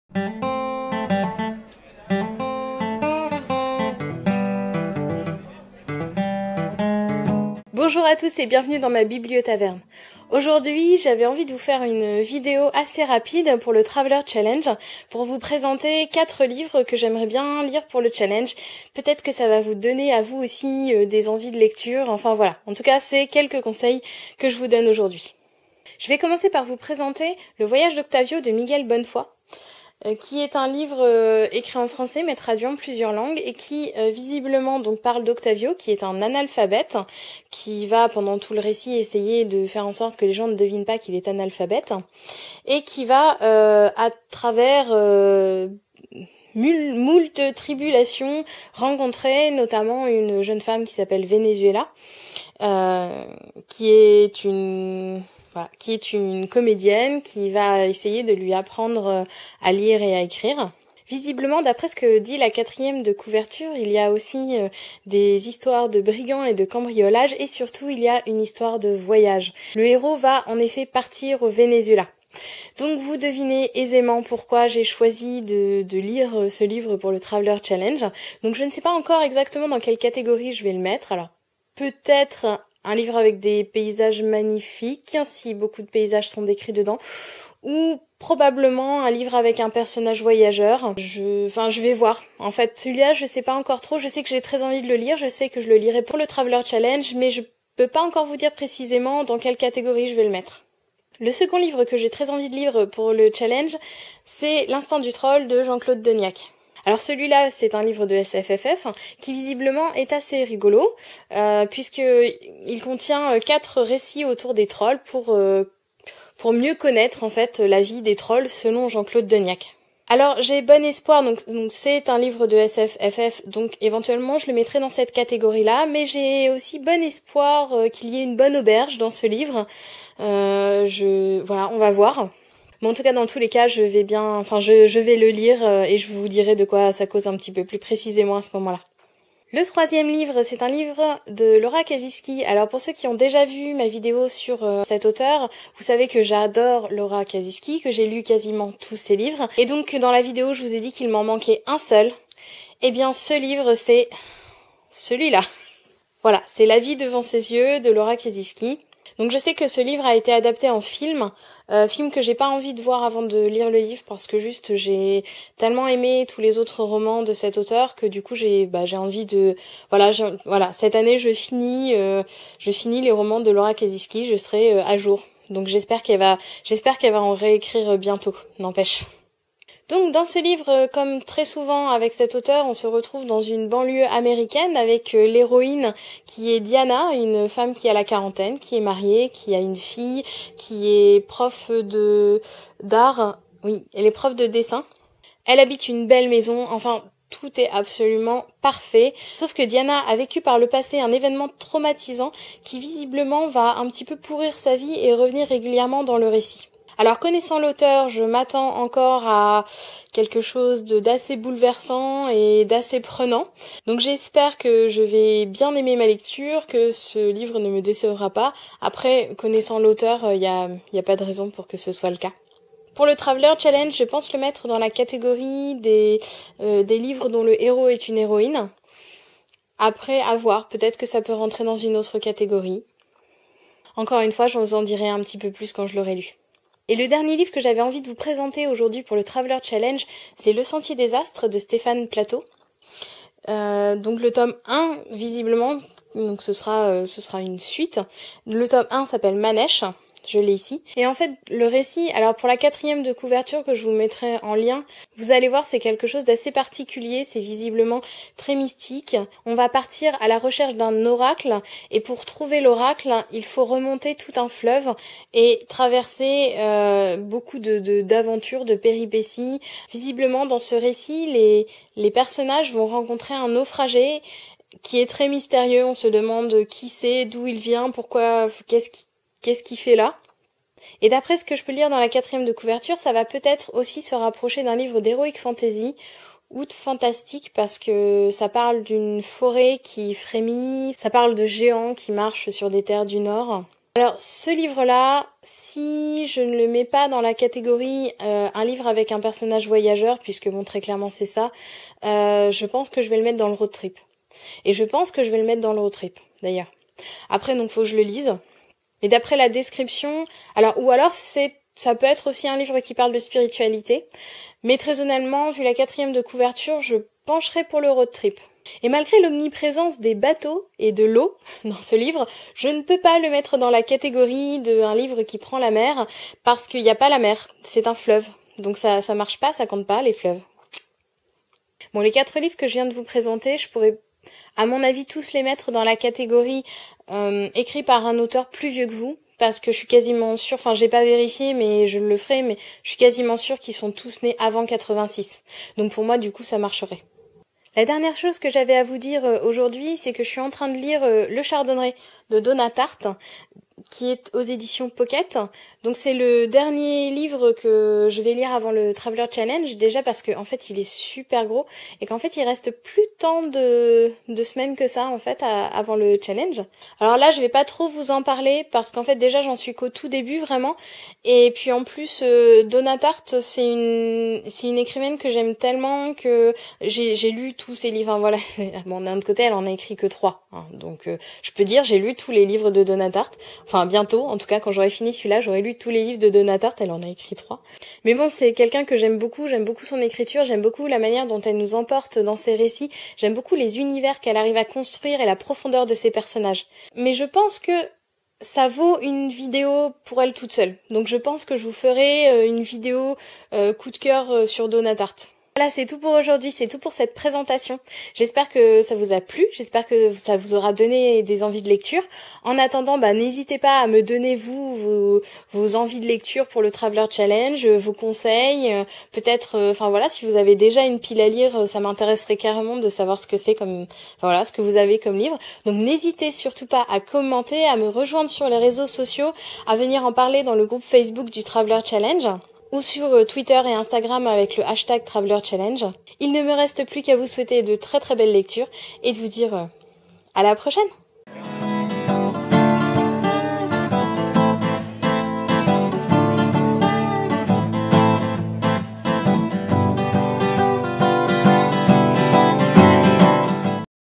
Traveler Challenge #2 - bookhaul
Déroulez !

Les livres présentés :

- "Le Voyage d'Octavio" de Miguel Bonnefoy (éditions Rivages)
- "L'instinct du Troll" de Jean-Claude Dunyach (édition L'Atalante)
- "La vie devant ses yeux" de Laura Kasischke (édition Le Livre de Poche)
- "Le sentier des astres T.1 - Manesh" de Stefan Platteau (édition J'ai Lu)
- "Le Chardonneret" de Donna Tartt (éditions Poket)